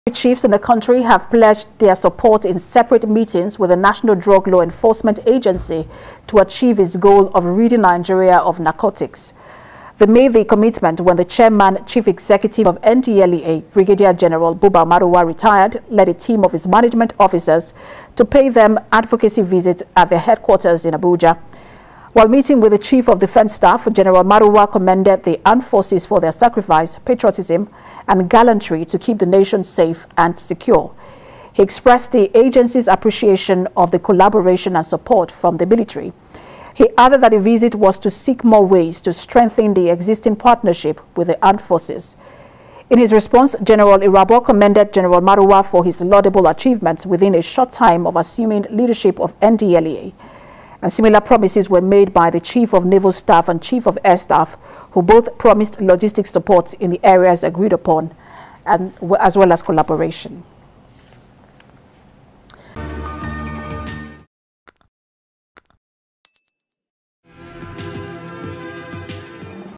Military Backs Drug War As Marwa Meets CDS, Army, Navy, Airforce Chiefs
Military chiefs in the country have pledged their support in separate meetings with the National Drug Law Enforcement Agency, to achieve its goal of ridding Nigeria of narcotics.